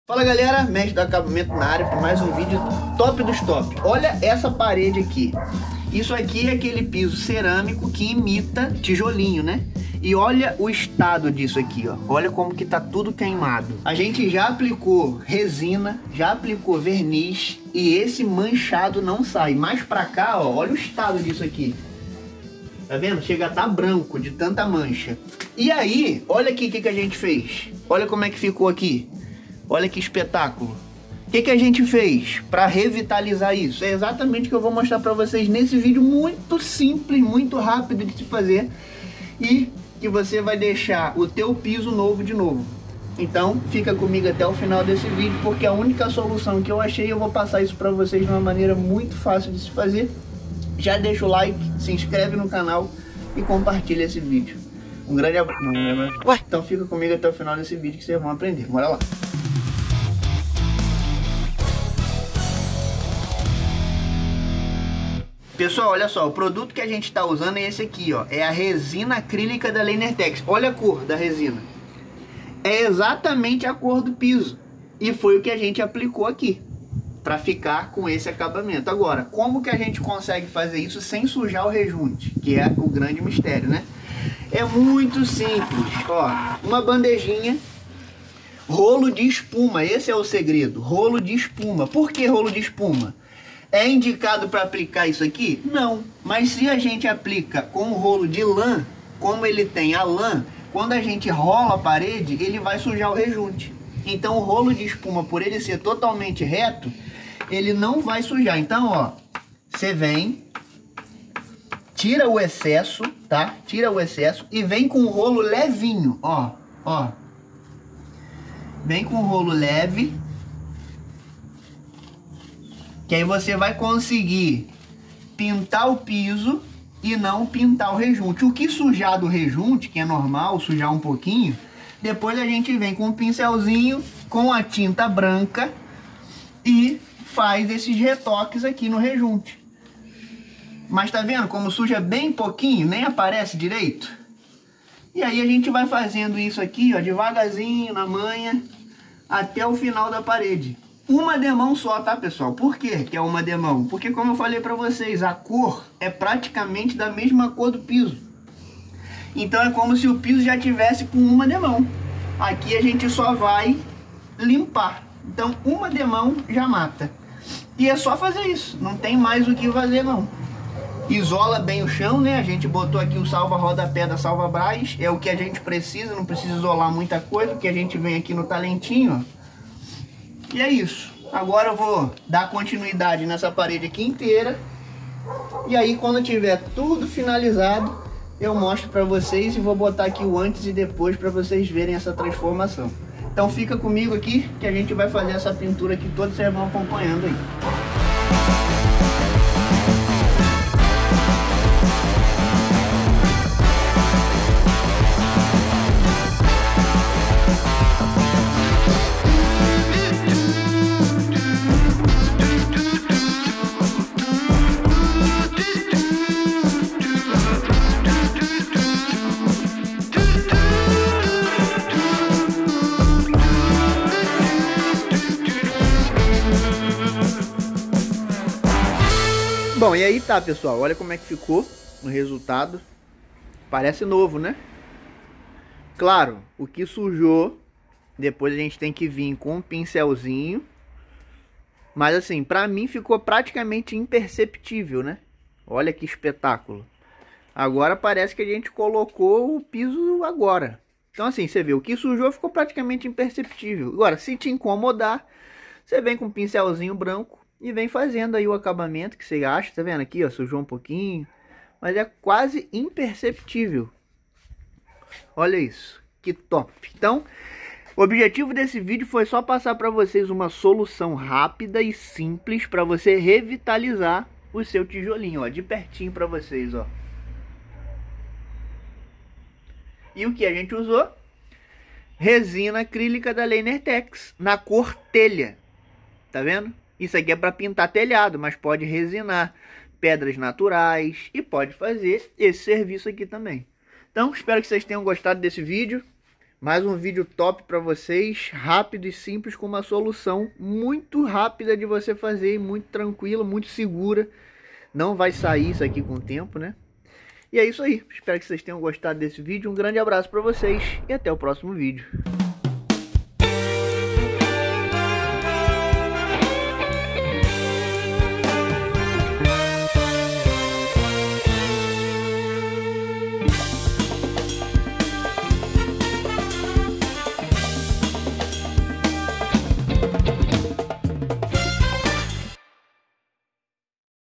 PORQUE EU NÃO DESCOBRI ISSO ANTES
FALA GALERA BELEZA? Estava com essa revestimento queimado com o tempo e por produtos químicos passados nele, já tinha aplicado verniz, resina e nada resolvia, até que na ultima tentativa achamos uma solução fantástica, aplicamos a resina na cor telha da Leinertex e conseguimos resolver, mas olha a dica top que eu passo no vídeo, já deixa seu like e se inscreva no canal.